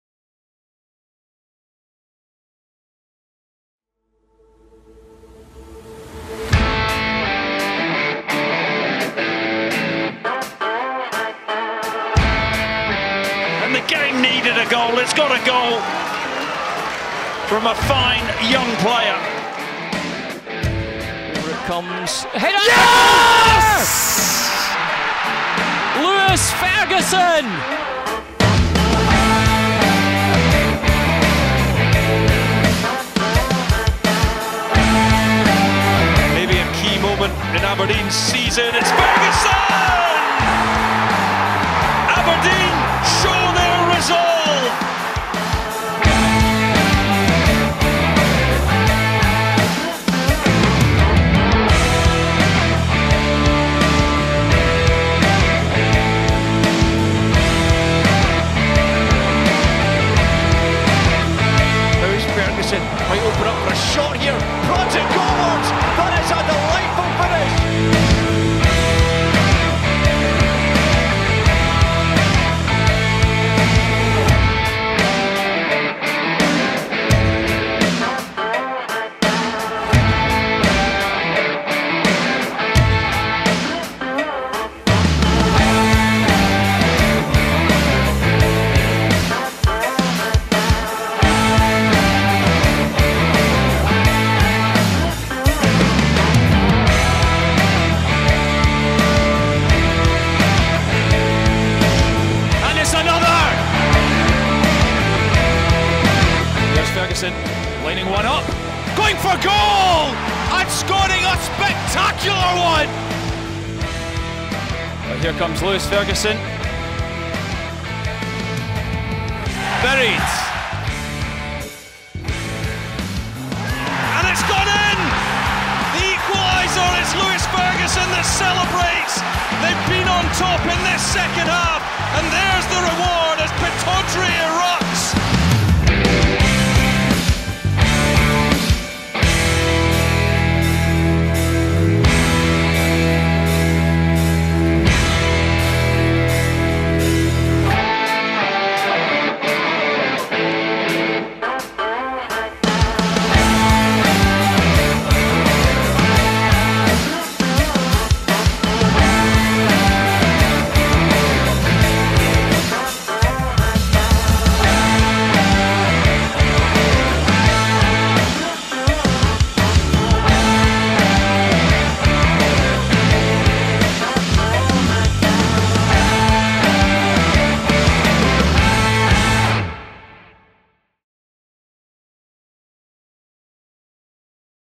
Watch Lewis Ferguson's goals for The Dons as he departs for Italy!
As Lewis departs for pastures new, take a look back at his goals for The Dons over the last 4 years.

Good luck in Italy Lewis!